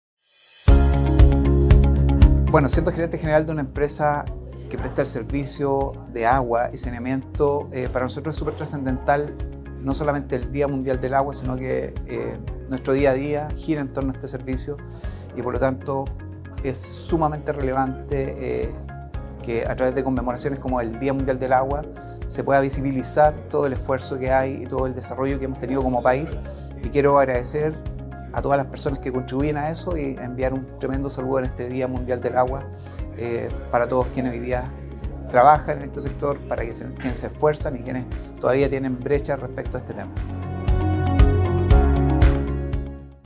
DíaDelAguaCRHIAM 2023 - Cristian Vergara, Gerente general ESSBIO
Les dejamos el saludo por el Día Mundial del Agua, del Gerente general de ESSBIO, Cristian Vergara.

Este año, esta efeméride tuvo como objetivo visibilizar y trabajar en pos del ODS 6 "Agua y Saneamiento" 💧

Para conocer más sobre el trabajo del Centro, visita nuestra página web